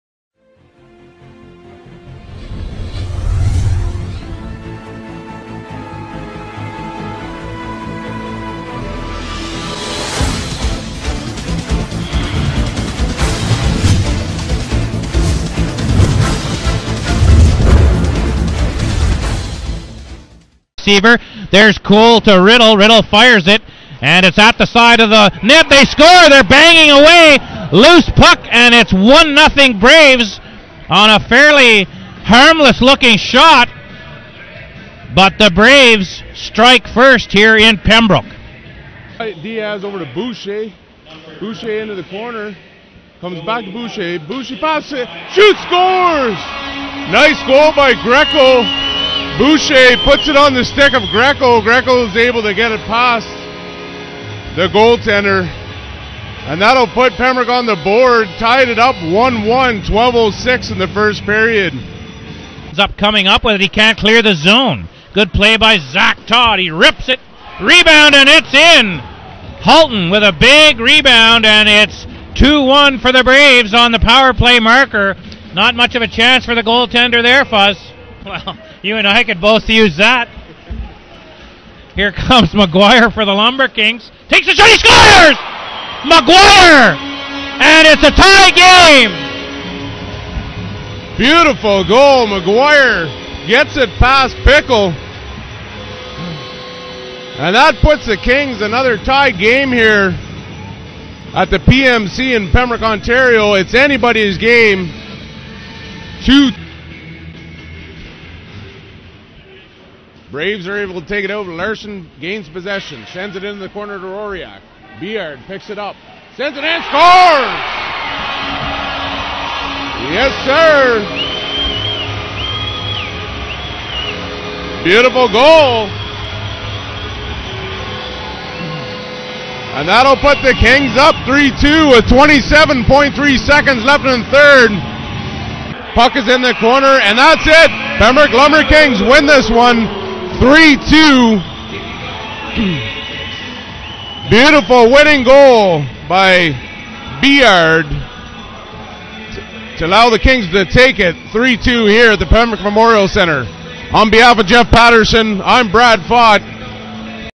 Braves at pembroke Feb 10, 2103
CCHL regular season action in Pembroke. Kings win 3 -2